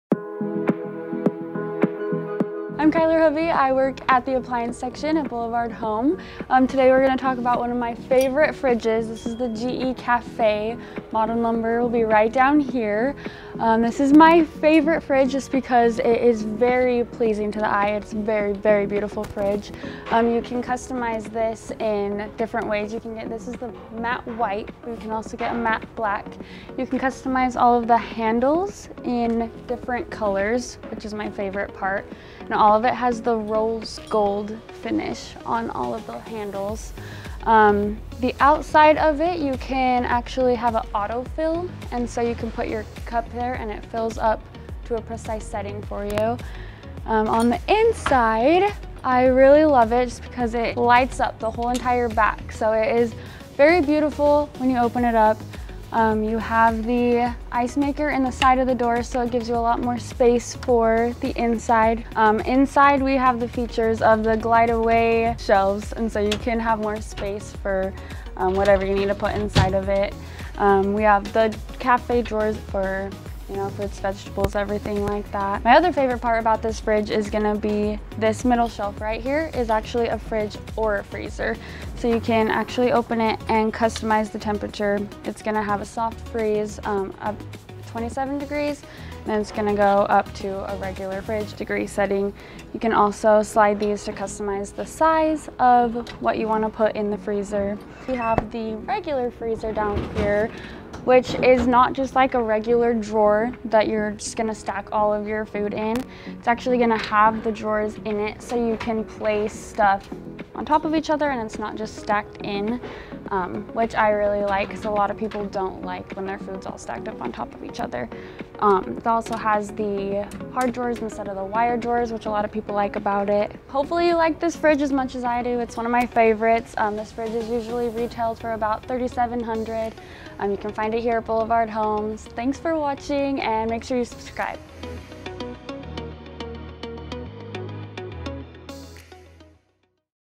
2021 Best Refrigerator - Model CVE28DPNW2
2021 Refrigerator Product Review!

Cafe Refrigerator Model: One of the best refrigerators for 2021.

CVE28DP4NW2 - Matte White
CVE28DP3ND1 - Matte Black
CVE28DP2NS1 - Stainless Steel

Watch as Kyler share with us why this Matte White finish model is her favorite fridge for 2021.

What makes is so unique?

1.  Cafe's Matte White and Matte Black finishing are truly amazing! Plus you can choose between 4 different handles to customize the kitchen to fit you! Plus you get the rose gold accents!

2.  The water dispenser! This dispenser has all the features we have come to love.  Cubed and Crushed Ice, Dispensing filtered water, but it also has this new feature called Auto Fill.  This fills your bottle up without any effort on your part.  The fridge senses how much liquid your bottle holds and fills it up.  Awesome right!!!

3. The interior of this fridge is crazy cool!! The LED back wall is one of a kind, plus you get the ice in the door option which saves space and super convenient.  There are also two humidity controlled drawers one to keep the humidity in and one to push the humidity out.  These are perfect to keep your food long term tasting and looking fresh which means it saves money! I can't talk enough about the inside of this fridge.  It also has heavy duty door bines which can be moved around based on your needs and it has a cool breakaway shelf adding to the convenience and enable you to add tall items in your fridge without removing a complete shelf.

4. Convertible Fridge Freezer Drawer: This is the perfect drawer for those unique one of a kind items like ice cream or bottle of wine which don't always go well in your standard freezer or refrigerator temperature.   With multiple temperature setting you have the flexibility to have the temperature down as low as 23 degrees for the perfect soft ice-cream or up to 42 degrees to keep your wine at
that perfect temperature.

5. This is the thing it like least about this fridge, but its the same issue that all French door refrigerators have and that is the ability to easily access items in the freezer. The Café French Doors does try to improve on this challenge by adding two additional compartments inside the freezer.  This does help in the organization of the freezer but it also eliminates some of the freezer space.

Bonus Takeaways:  Prices start around $3,700 this fridge. It is  on the higher end of French doors.   This may put it out of reach for those on a budget, but if you appreciate the features it is well worth it.  Last off this fridge can be connected to the Wi-Fi at your home. There are not many amazing benefits from it yet, but simply having the ability to be notified if your fridge is left open or that there is a service issue could save you $100's in food spoilage or future service issues.

Overall on a scale of 0-10 with 10 being the best this new French door fridge by Café in our opinion is a 8.5.   There are some amazing new features that are unique in the industry, but we feel like because of the freezer there could still be some improvements.

Fridge Specs:

27.8 Cu. Ft. Smart 4-Door French-Door Refrigerator
Fresh Food Capacity - 15.62 cu ft
Freezer Capacity - 8.57 cu ft
Convertible Drawer Capacity - 3.62 cu ft

* Parts and Labor Warranty:  Limited 5 year sealed refrigerating system, Limited 1 year entire appliance
* Leveling - 2 point front adjustable feet
* 4 glass spit with 1 quick space shelf (Breakaway Shelf)
 * This is a freestanding refrigerator
* 6 total door bins (2 gallon storage, 1 adjustable)
* Icemaker in the door.
* Convertible Drawer comes with 6 temperature settings. (Soft Freeze, Meats, Beverages, Snacks, Wine, Lock)
* LED Lighting
* TwinChill Evaporators
* Door Alarm, Water filter indicator light, LED Dispenser Light, Hands-Free AutoFIll.